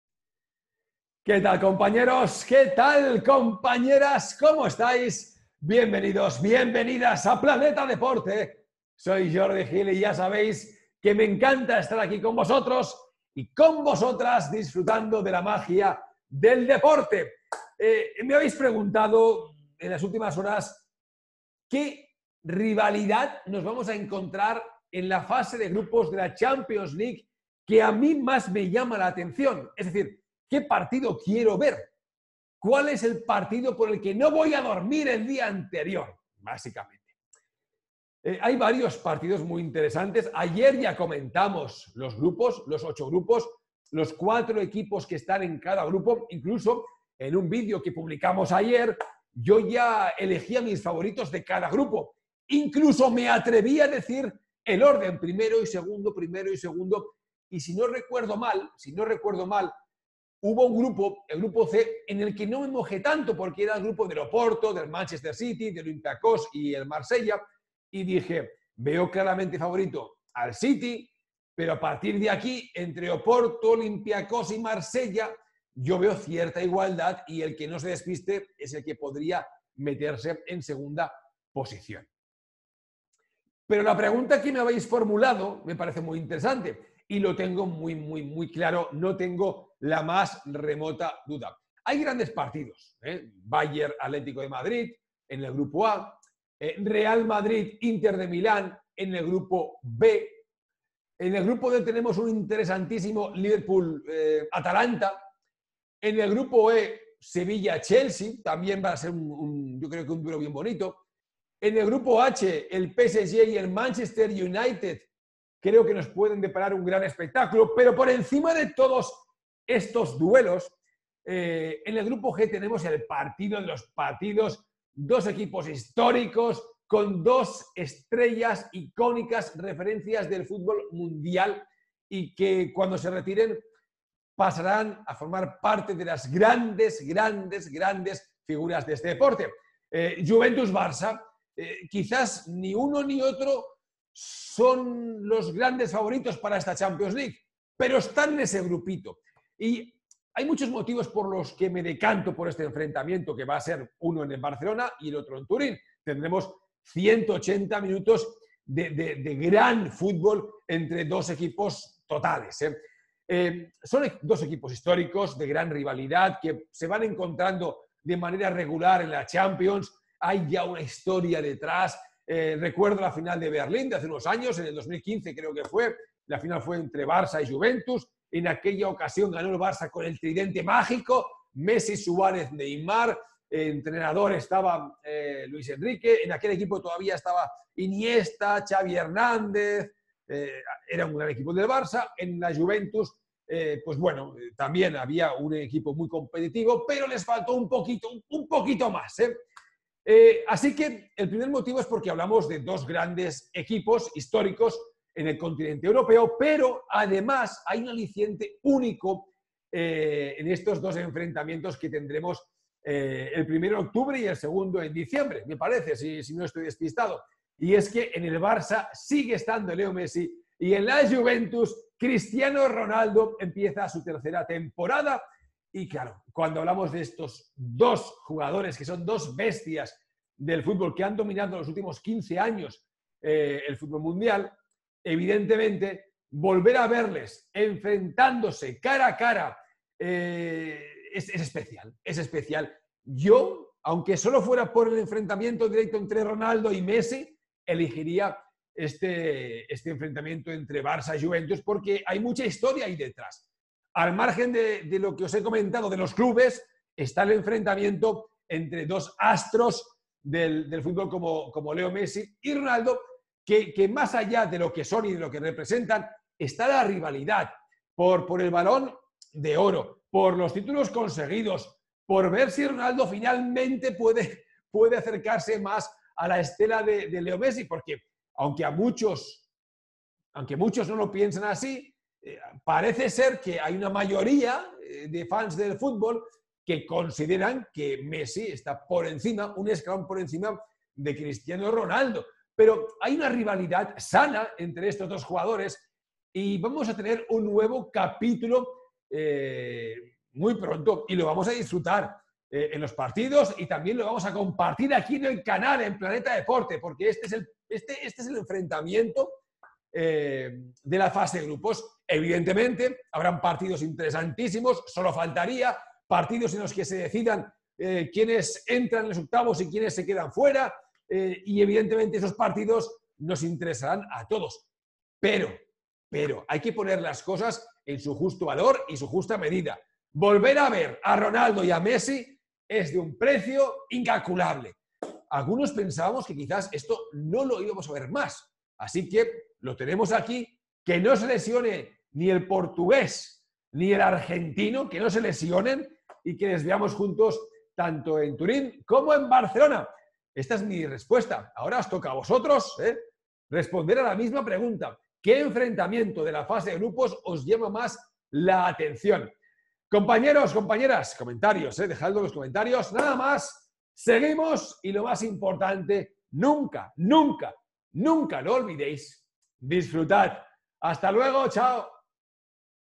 El DUELO entre MESSI y RONALDO es lo más atractivo de la liguilla de la CHAMPIONS LEAGUE
El sorteo de la fase de grupos de la Liga de Campeones deparó este 1 de octubre de 2020, en Ginebra, que el Barcelona de Lionel Messi y la Juventus de Cristiano Ronaldo formen parte del mismo grupo, el G, mientras que el campeón Bayern Múnich tendrá como principal rival en el A al Atlético de Madrid. El encuentro entre Barça y Juventus será el primero en el que Cristiano se enfrontará al Barcelona tras abandonar al Real Madrid.

Messi ha ganado 34 títulos con el FC Barcelona y dos con la de la selección argentina (Mundial sub-20 en 2005 y el oro olímpico, en 2008). Cristiano Ronaldo, por su parte, ganó 16 con el Real Madrid, 8 con el Manchester United, 3 con la Juventus y 1 con el Sporting CP. Con la selección portuguesa sumó otros dos (Eurocopa 2016 y Europa Nations League 2019).